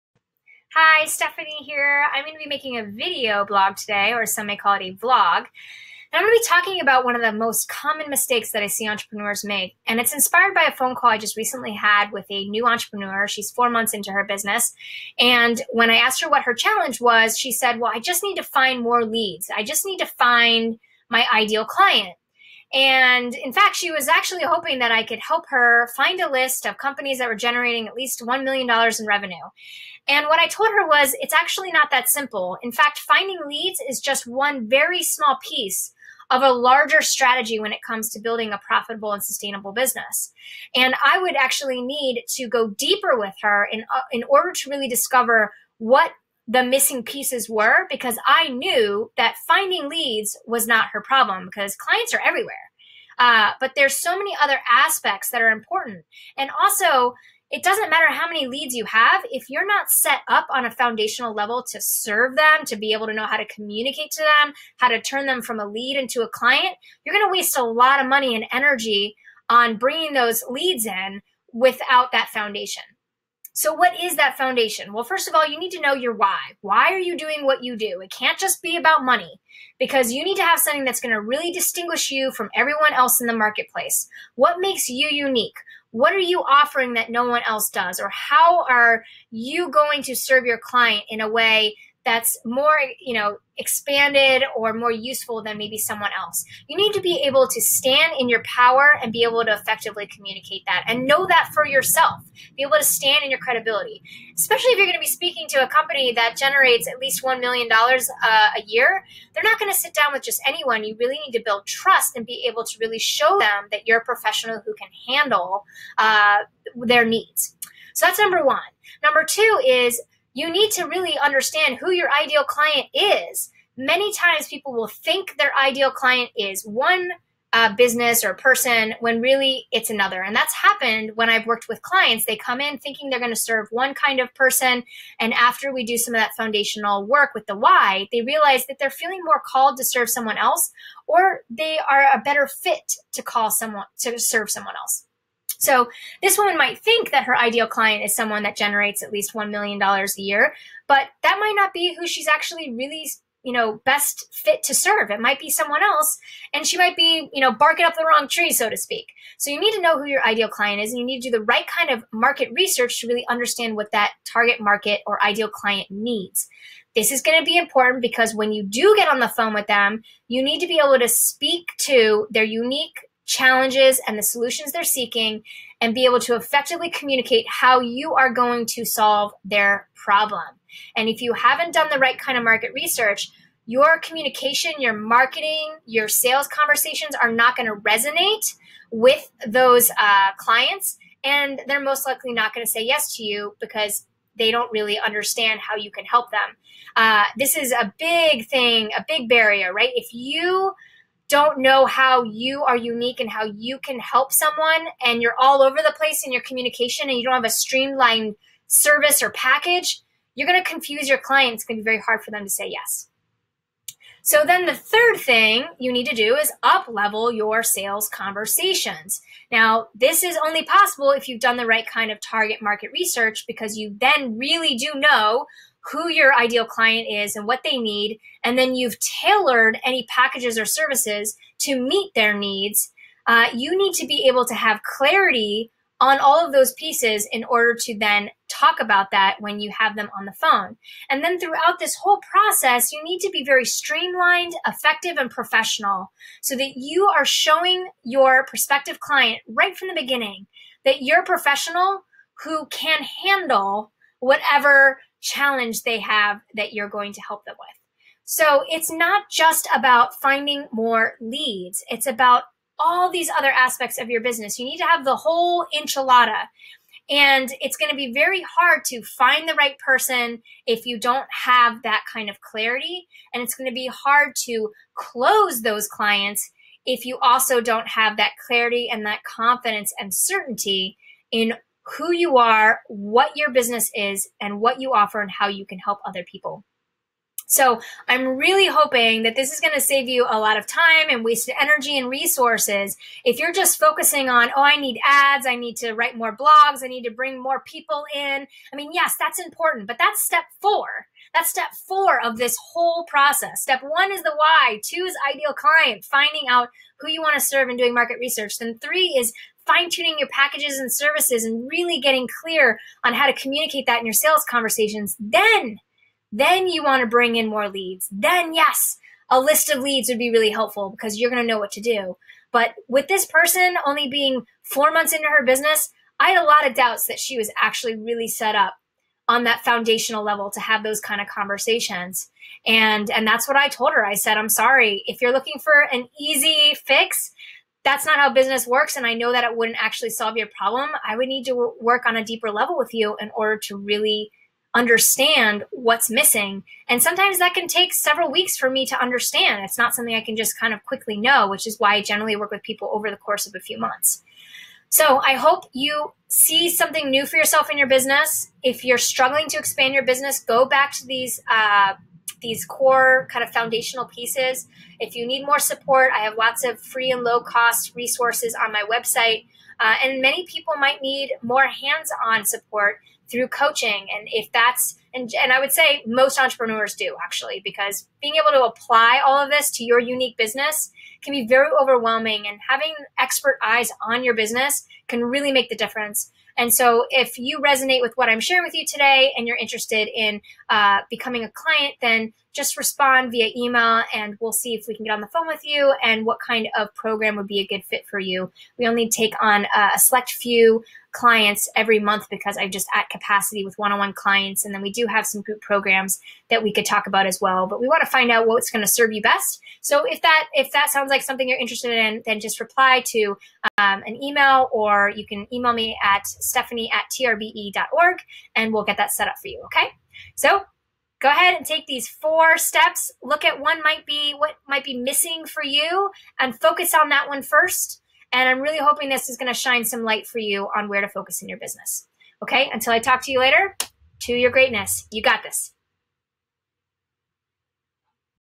The BIG Mistake Entrepreneurs Make
I get really frustrated with all the BAD business advice out there and I talk about one of the biggest mistakes entrepreneurs make in this video.  Are you focusing on a tiny aspect of your business and ignoring the bigger picture?  Watch this video to learn more.